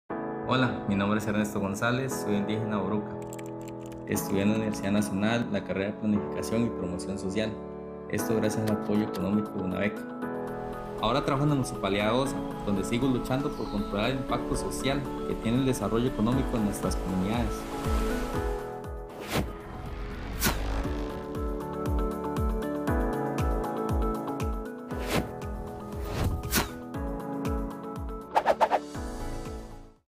Fortalezcamos la U Pública: Ernesto González
Más del 64% de las personas graduadas en Universidades Públicas de Costa Rica provienen de hogares donde los padres no tienen título universitario.

-Fortalezcamos la U Pública y sigamos construyendo país-